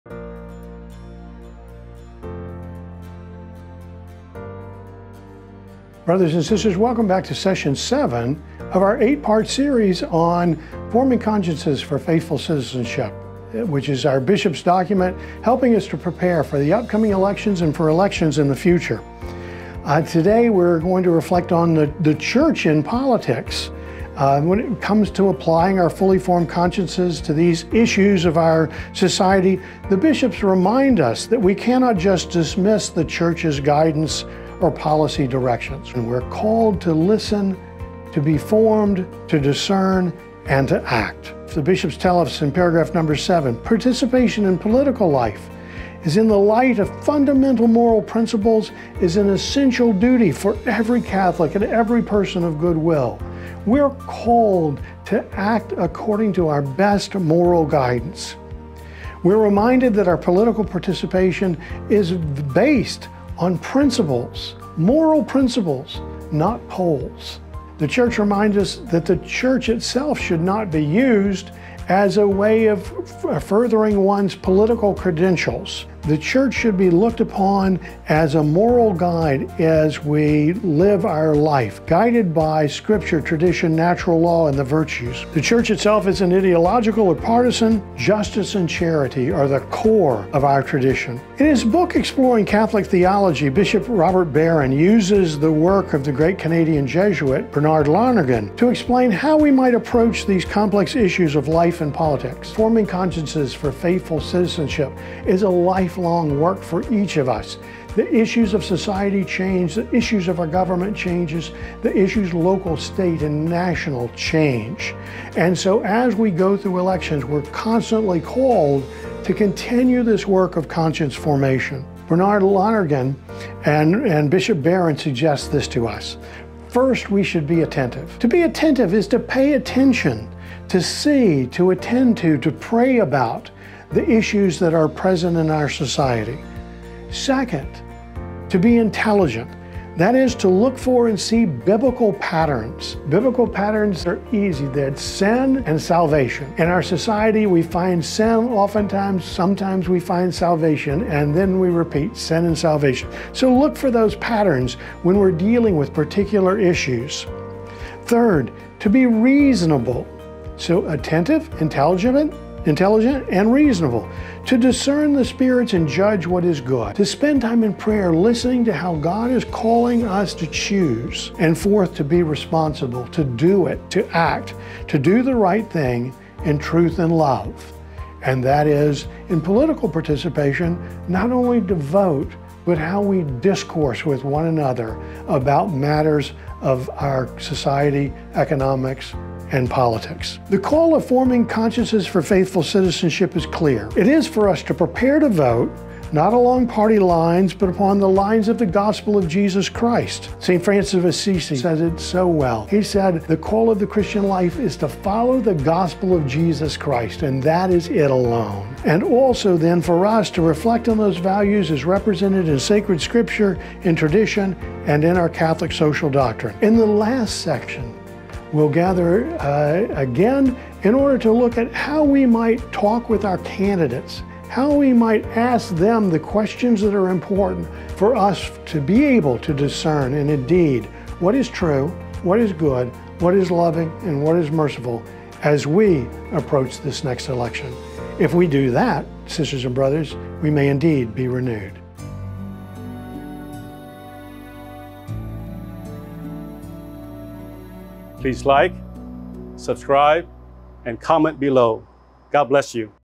Renewed Faithful Citizenship Episode 7: The Church and Politics
This session explores the Catholic Church’s guidance on political participation, and the importance of applying a well-formed conscience to societal issues based on moral principles, Scripture, and tradition.